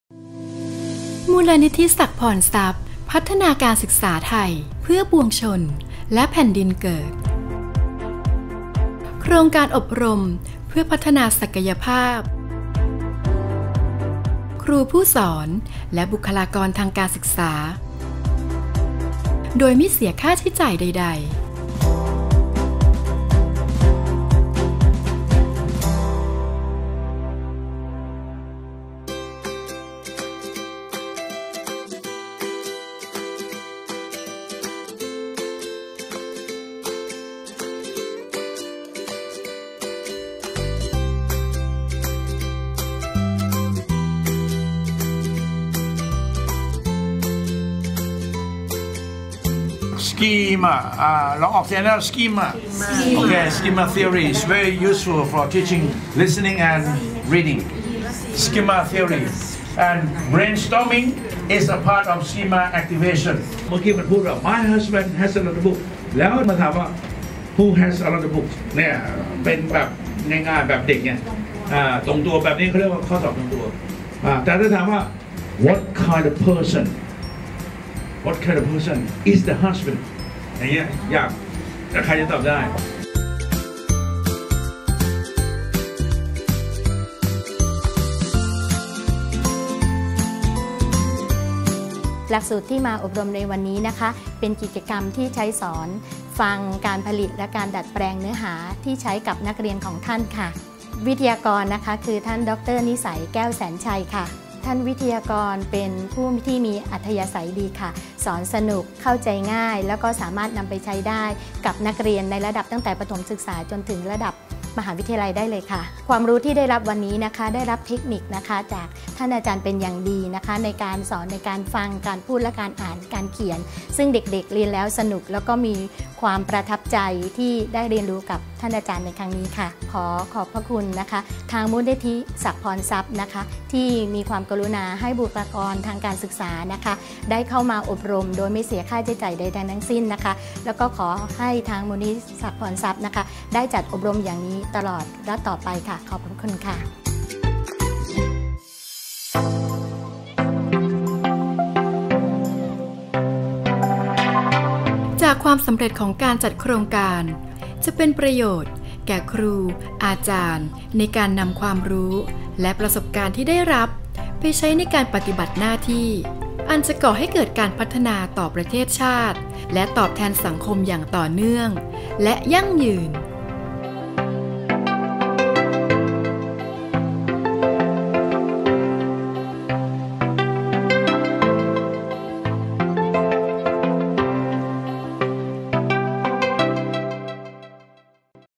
กิจกรรมที่ใช้สอนฟัง: การผลิตและการดัดแปลงเนื้อหาให้ใช้กับนักเรียนของท่าน วันที่ 28 พฤษภาคม 2560
หลักสูตร กิจกรรมที่ใช้สอนฟัง: การผลิตและการดัดแปลงเนื้อหาให้ใช้กับนักเรียนของท่าน (Activities for Teaching Listening How to create your own and adapt materials for your students)
วิทยากร : ดร.นิสัย แก้วแสนไชย
วันที่ 28 พฤษภาคม 2560 เวลา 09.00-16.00 น.
สถานที่อบรม : สำนักงานมูลนิธิศักดิ์พรทรัพย์